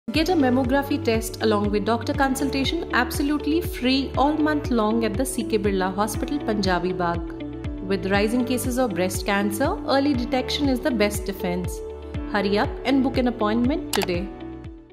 Free Mammography & Review offer at CK Birla Hospital Delhi
Help is at hand, at the CK Birla Hospital, Punjabi Bagh. This October we are doing a Breast Cancer Awareness Drive, where we are offering upto 100% discount on our Breast Cancer Early Detection Package. This early bird offer includes Free Consultation by the leading Oncologists and Breast Cancer Specialists in Delhi NCR and a Free Mammography that will help in the early detection of breast cancer.

This offer is available to all the women who walk-in to our hospital all month, free of charge to ensure that more and more women across age groups are aware of the disease and its symptoms. Women are the backbone of our society, and it is our duty to protect them from unforeseen dangers, for together we canbeatcancer.

     Message: WhatsApp us!